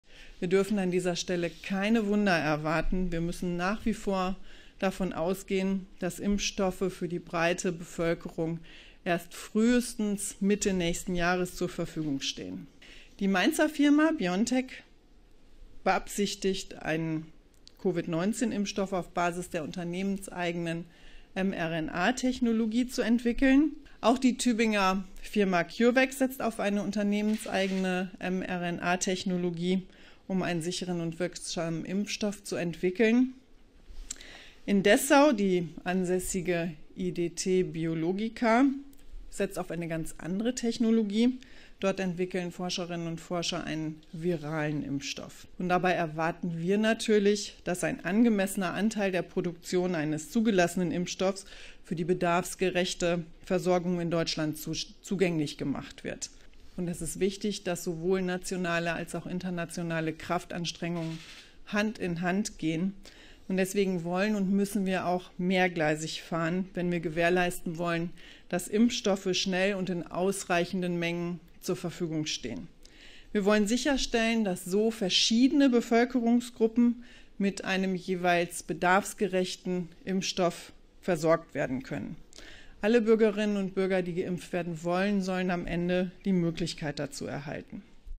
Karliczek: Corona-Impfstoff frühestens Mitte 2021
Alle Bürgerinnen und Bürger, die geimpft werden wollten, sollten am Ende die Möglichkeit dazu erhalten, sagte Bundesforschungsministerin Anja Karliczek. Ein Impfstoff erwarte sie allerdings erst im kommenden Jahr.